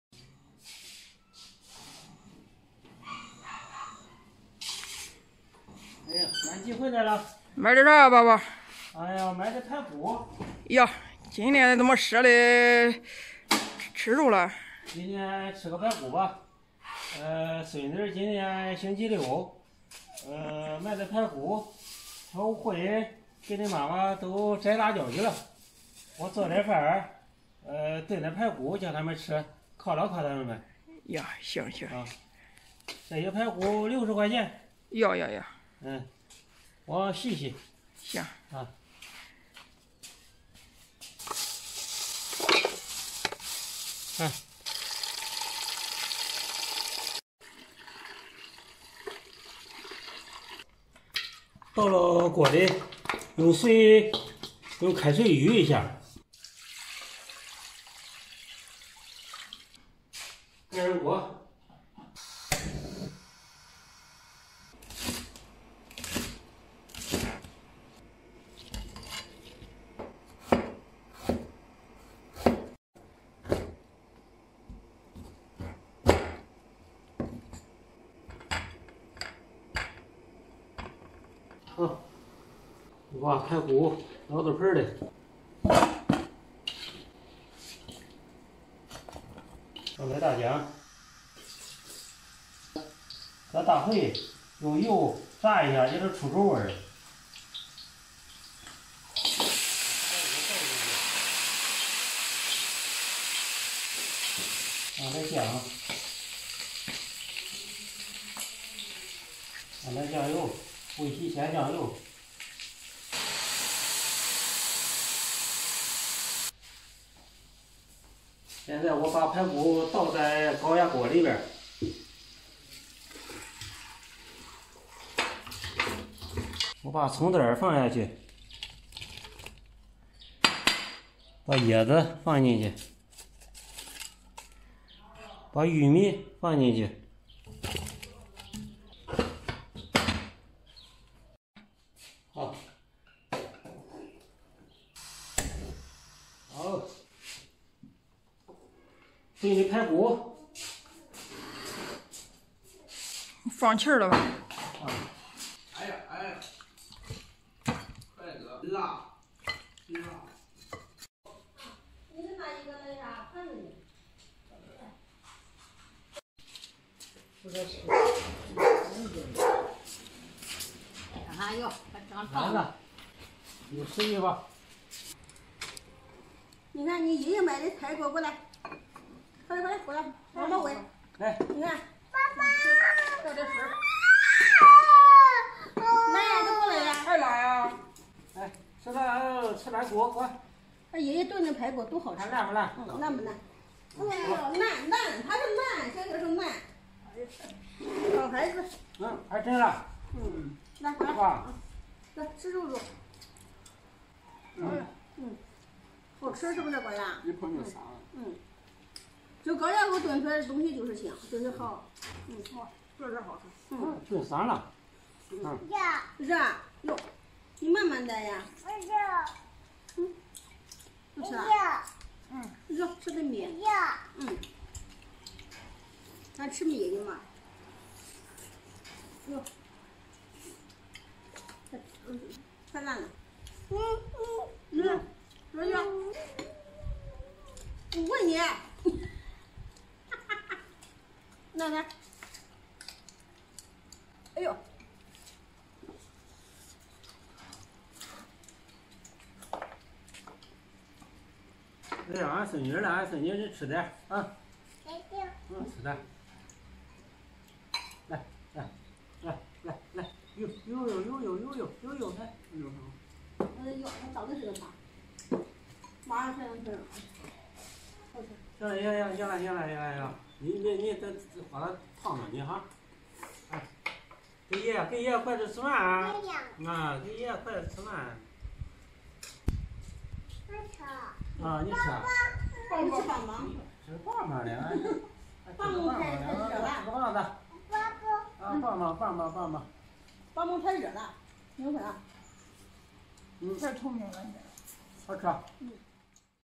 爺爺下廚做大餐，一盆玉米燉排骨香得很，犒勞一下婆媳倆【我是趙姐】
我们强调了视频内容的合规性，绝对没有任何不良引导或危险行为。
婆婆兒媳下地摘辣椒，公公下廚改善伙食犒勞婆媳倆，兒孫跟著沾光